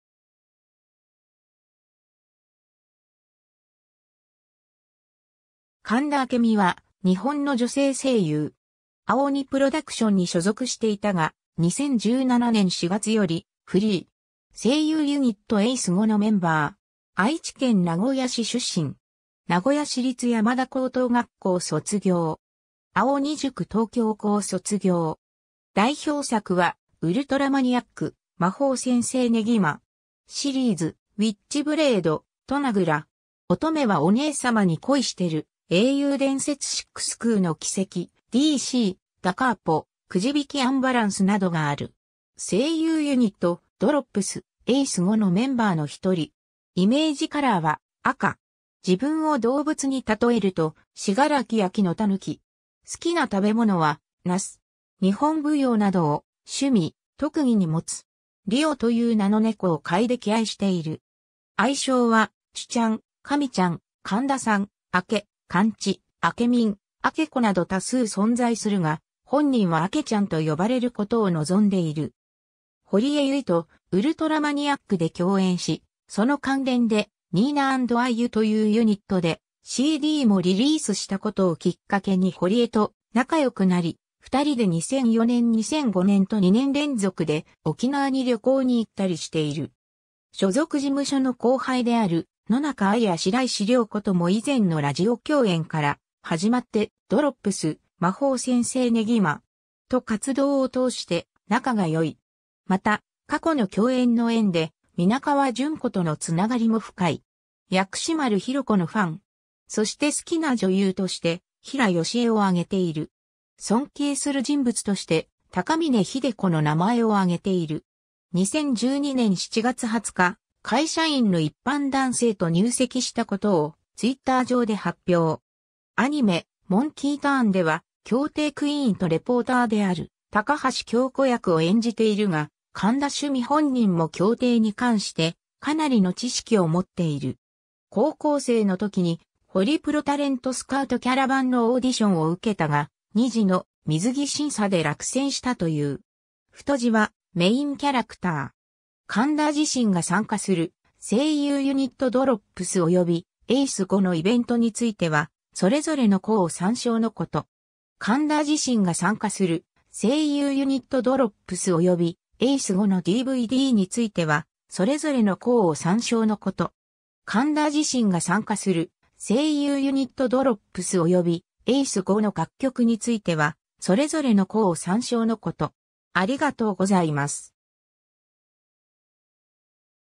神田朱未
神田 朱未（かんだ あけみ、1978年11月10日 - ）は、日本の女性声優。
青二プロダクションに所属していたが、2017年4月よりフリー。
声優ユニット・Aice5のメンバー。
愛知県名古屋市出身。
名古屋市立山田高等学校卒業。
青二塾東京校（19期生）卒業。
 代表作は『ウルトラマニアック』（佐倉仁菜/ニナ・サクレイル）、『魔法先生ネギま!』シリーズ（神楽坂明日菜）、『ウィッチブレイド』（天羽梨穂子）、『となグラ!』（有坂香月）、『乙女はお姉さまに恋してる』（周防院奏）、『英雄伝説VI 空の軌跡』（エステル・ブライト）、『D.C. 〜ダ・カーポ〜』（天枷美春）、『くじびきアンバランス』（秋山時乃）などがある。
 声優ユニット「DROPS」「Aice5」のメンバーの1人。
イメージカラーは「赤」。
自分を動物に例えると信楽焼の狸。
好きな食べ物は、茄子。
日本舞踊などを趣味・特技に持つ。
「リオ」という名の猫を飼い溺愛している。
愛称は朱ちゃん（あけちゃん）、神ちゃん、神田さん（カンださん）、アケ、かんち（カンチ）、あけみん、あけこなど多数存在するが、本人はあけちゃんと呼ばれることを望んでいる。
 堀江由衣と『ウルトラマニアック』で共演し、その関連で「Nina & Ayu」というユニットでCDもリリースしたことをきっかけに堀江と仲良くなり、2人で2004年・2005年と2年連続で沖縄に旅行に行ったりしている。
 所属事務所の後輩である野中藍や白石涼子とも以前のラジオ共演（『スタチャ情報発信番組 ホレぼれ。』や『ESアワー ラジヲのおじかん』など）から始まってDROPS・『魔法先生ネギま!』と活動を通して仲がよい。
また、過去の共演（『ときめきメモリアル3』）の縁で、皆川純子との繋がりも深い。
 薬師丸ひろ子のファン。
そして好きな女優として、平淑恵を挙げている。
尊敬する人物として、高峰秀子の名前を挙げている。
 2012年7月20日、会社員の一般男性と入籍したことをTwitter上で発表。
 アニメ『モンキーターン』では競艇クイーンとレポーターである高橋香子役を演じているが、神田朱未本人も競艇に関してかなりの知識を持っている。
 高校生の時に「ホリプロタレントスカウトキャラバン」のオーディションを受けたが、2次の水着審査で落選したという。
 ※ 神田自身が参加する声優ユニットDROPS及びAice5のイベントについては、それぞれの項を参照のこと。
 ※ 神田自身が参加する声優ユニットDROPS及びAice5のDVDについては、それぞれの項を参照のこと。
 ※ 神田自身が参加する声優ユニットDROPS及びAice5の楽曲については、それぞれの項を参照のこと。